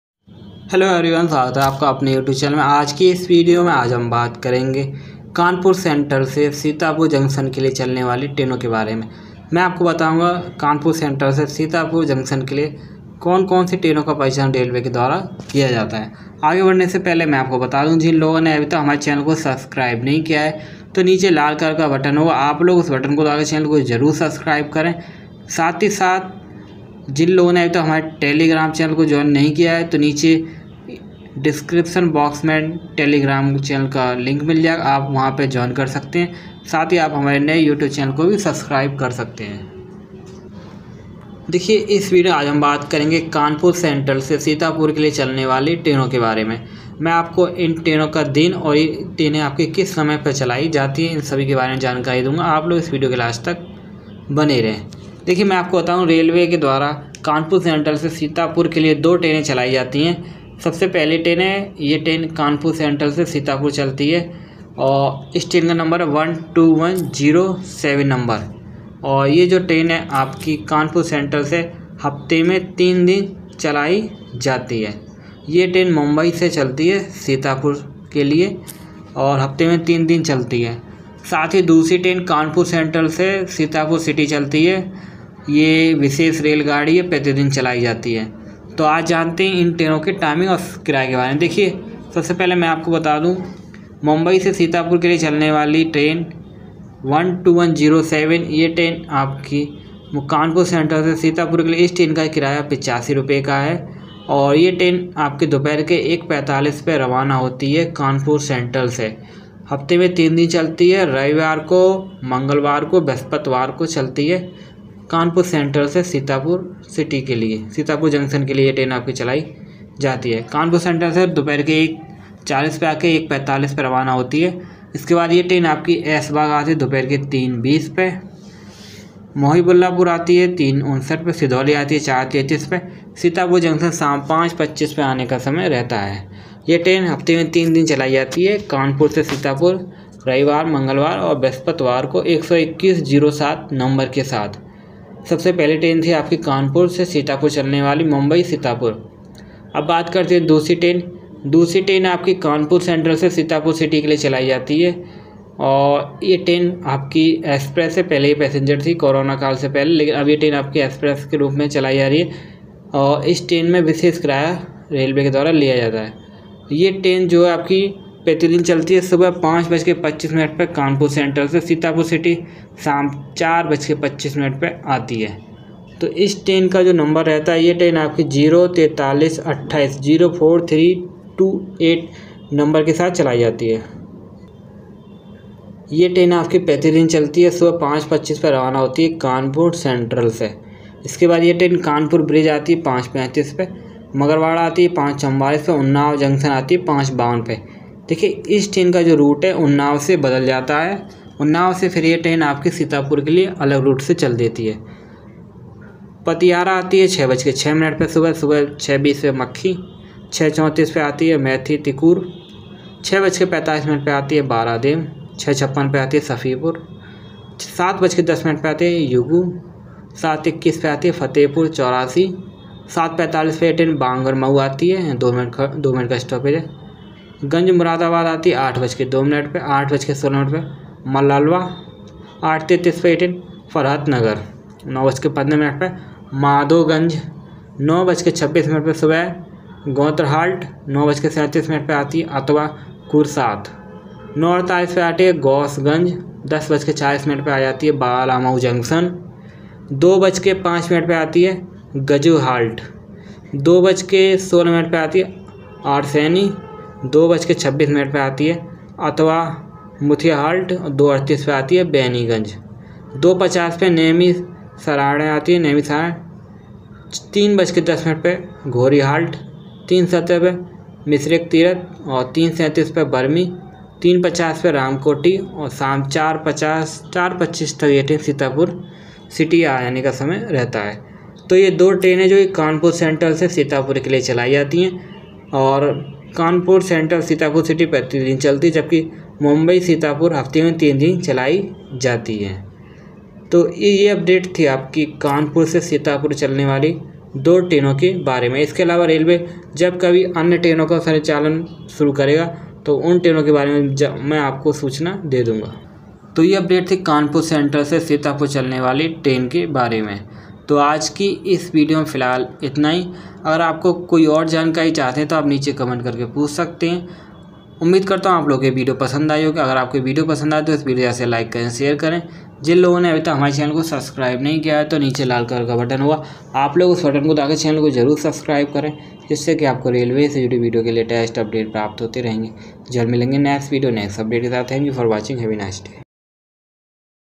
कानपुर से सीतापुर सिटी के लिए ट्रेन। kanpur se sitapur train
hello friends,🙏
 आज हम बात करेंगे कानपुर से सीतापुर के लिए चलने वाली सभी ट्रेन के बारे में।  संपूर्ण जानकारी के लिए इस वीडियो को पूरा देखें🙏